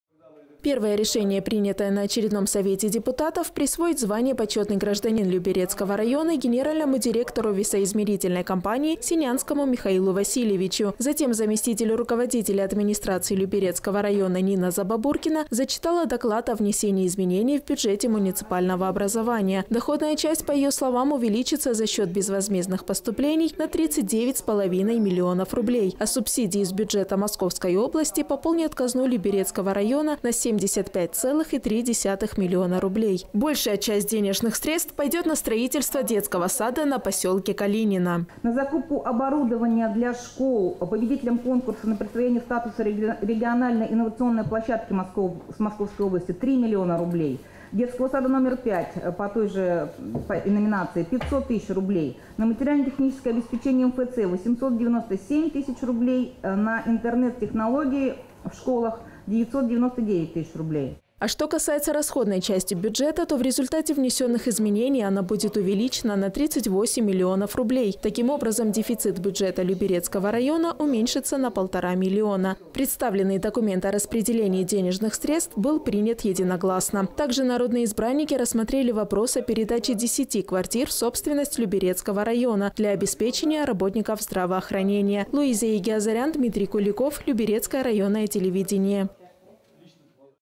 Заседание совета депутатов люберецкого района
Первое решение, принятое на очередном Совете депутатов – присвоить звание «почетный гражданин Люберецкого района» генеральному директору весоизмерительной компании Сенянскому Михаилу Васильевичу.  Затем заместитель руководителя администрации Люберецкого района Нина Забабуркина зачитала доклад о внесении изменений в бюджете муниципального образования. Доходная часть, по ее словам, увеличатся за счет безвозмездных поступлений на 39, 5 миллионов рублей. А субсидии из бюджета Московской области пополнят казну Люберецкого района на 75,3 миллиона рублей. Большая часть денежных средств пойдет на строительство детского сада в поселке Калинина.

Нина Забабуркина, заместитель Руководителя администрации Люберецкого района:
"На закупку оборудования для школ-победителей конкурса на присвоение статуса регионально-инновационной площадки МО – 3 млн. рублей, детского сада №5, по той же номинации – 500 тысяч рублей . На материально-техническое обеспечение МФЦ – 897 тысяч рублей, на интернет технологии в школах- 999 тысяч рублей".

А что касается расходной части бюджета, то в результате внесенных изменений она будет увеличена на 38 миллионов рублей. Таким образом, дефицит бюджета Люберецкого района уменьшится на 1,5 миллиона. Представленный документ о распределении денежных средств был принят единогласно. Также народные избранники рассмотрели вопрос о передаче 10 квартир в собственность Люберецкого района для обеспечения работников здравоохранения.